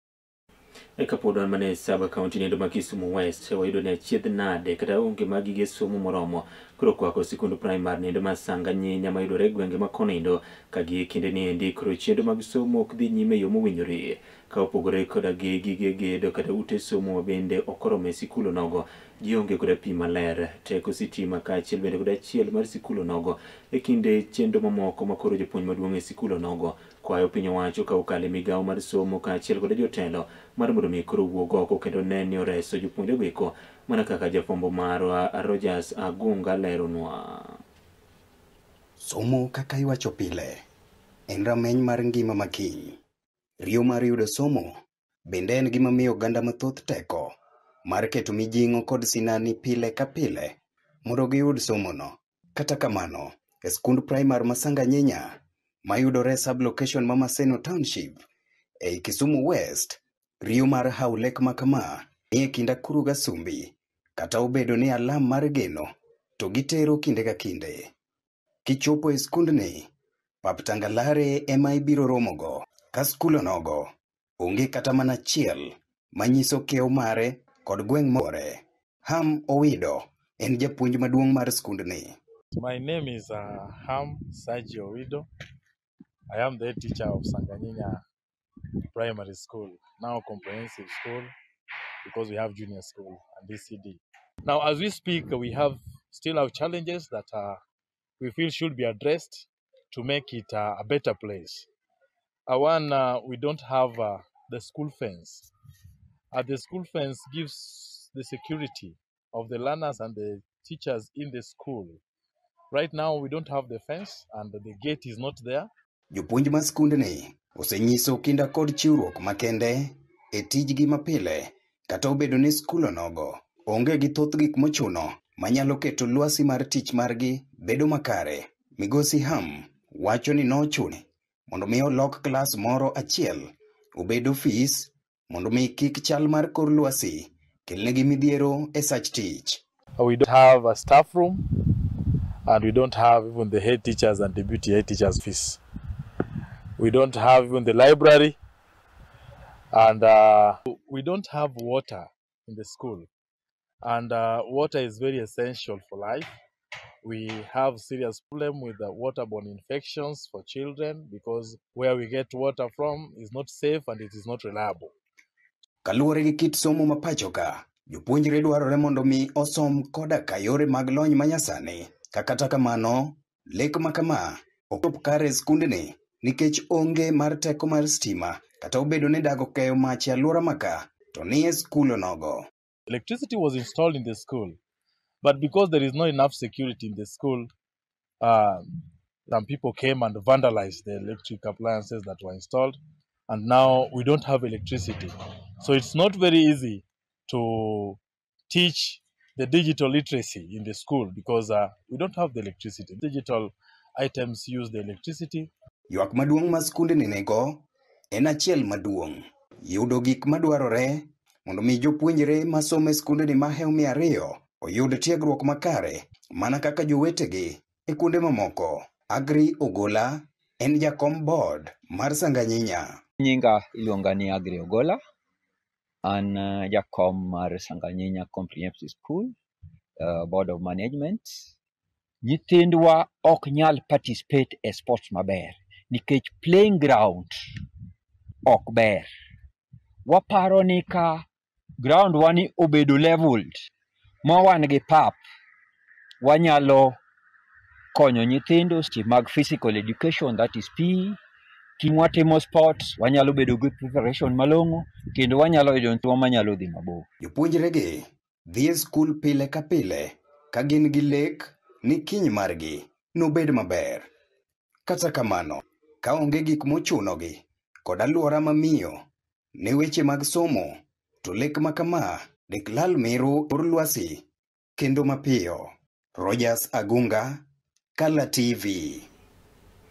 RIYO MAR SOMO SANGANYINYA
Midhiero mag onge gik mochuno thiro chenro mag somo Sanganyiya ei Maseno Township.